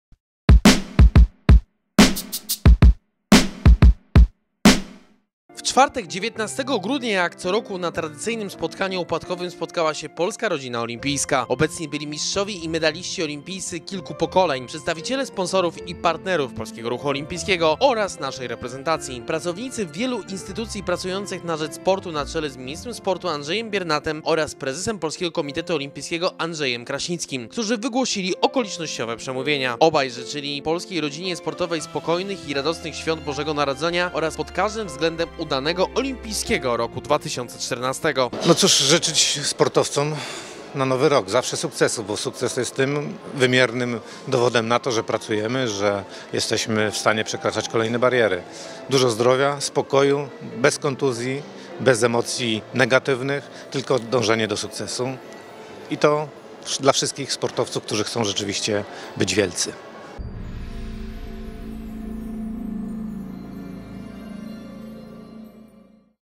Świąteczne spotkanie rodziny olimpijskiej
W czwartek, 19 grudnia, jak co roku na tradycyjnym spotkaniu opłatkowym spotkała się Polska Rodzina Olimpijska.